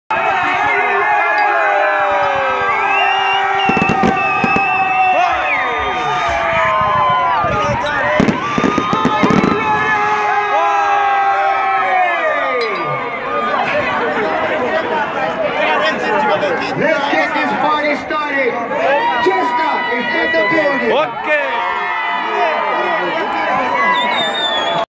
MTN COLOR FEST 2019
This was at the annual 2019 MTN COLOR FEST IN Zambia at the Levy Mwanawasa Stadium